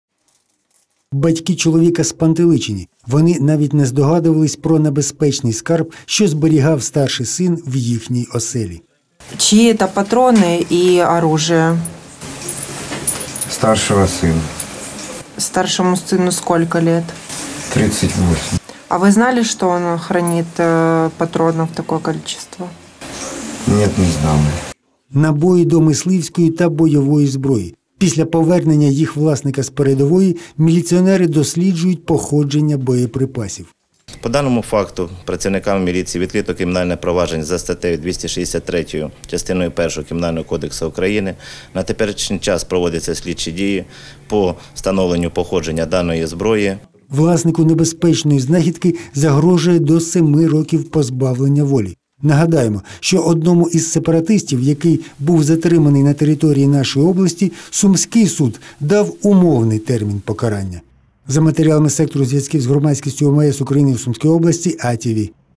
Правоохоронці виявили 600 набоїв різного калібру для стрілецької зброї
600 набоїв різного калібру для стрілецької зброї та ручну гранату виявили правоохоронці в одній з квартир Конотопа. Ймовірно, усе це привіз учасник АТО під час своєї короткострокової відпустки. Батьки навіть і не здогадувались - що зберігається в кімнаті сина.